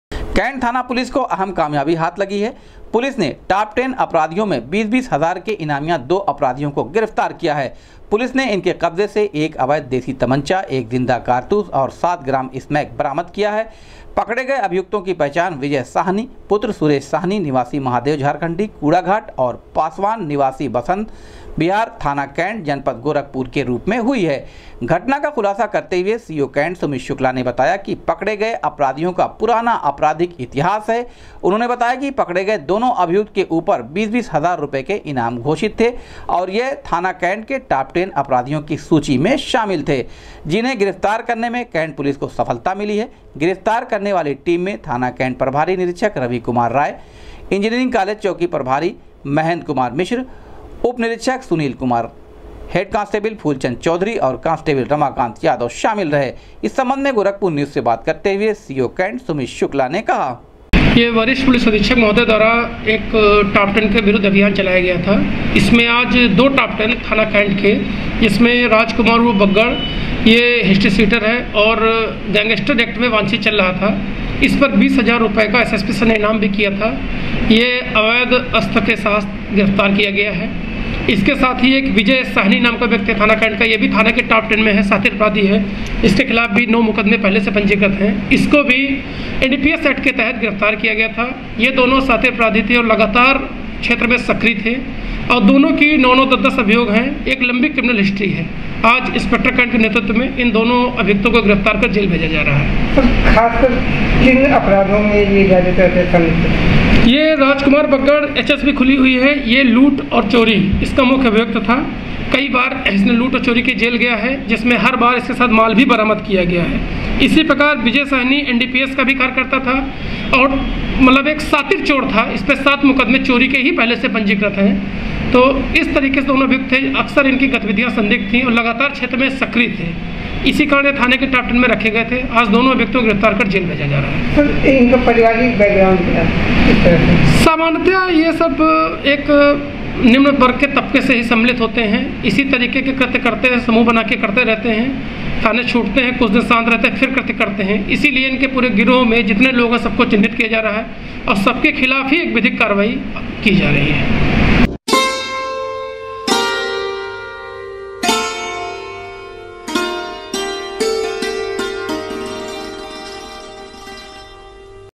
कैण्ट पुलिस ने टॉप टेन अपराधियों की सूची में शामिल 20-20 हजार रुपए के दो ईनामियां शातिर अपराधियों को
कैण्ट पुलिस ने टॉप टेन अपराधियों की सूची में शामिल 20-20 हजार रुपए के दो ईनामियां शातिर अपराधियों को किया गिरफ्तार, इनके कब्जे से एक अवैध देसी तमंचा एक जिंदा कारतूस और 7 ग्राम स्मैक किया बरामद, सीओ कैण्ट सुमित शुक्ला ने किया खुलासा